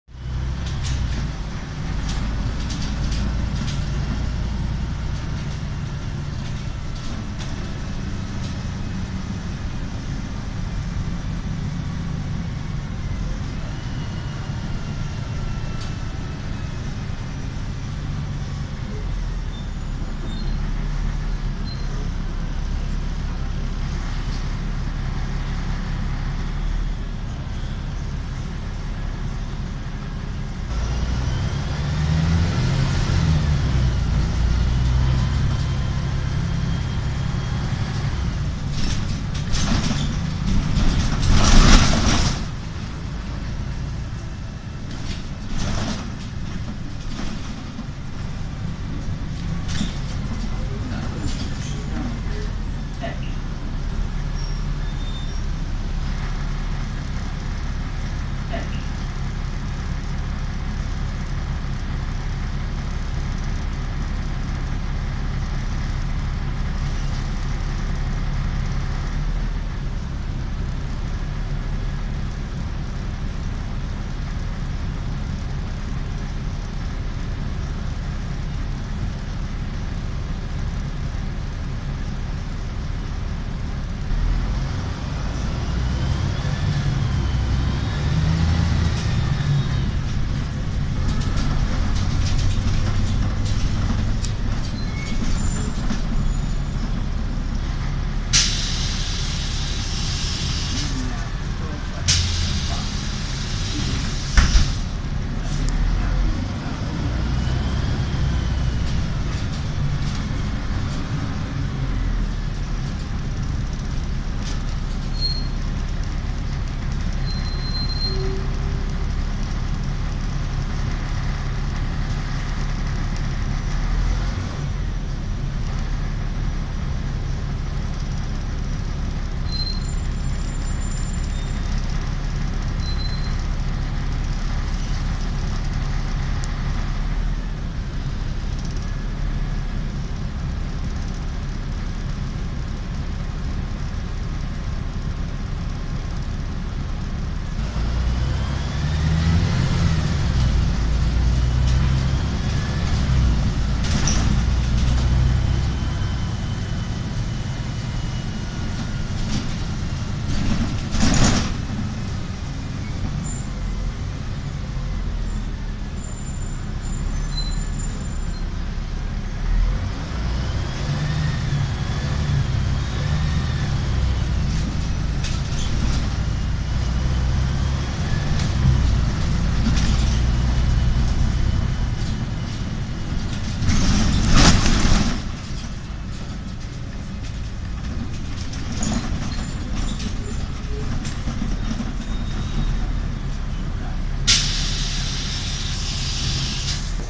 Linia 122 - [*] Volvo 7000 #7011 (MPK Wrocław)
Przystanki:
1. Dworzec Autobusowy
2. EPI
3. Gwiaździsta NŻ

Linia 122 w kierunku: Zajezdnia ul. Obornicka

[Krótkie dane pojazdu]
Rok produkcji: 2003
Rok wycofania: 2020
Silnik: Volvo D7C250
Skrzynia biegów: Voith D854.3 (4 biegi)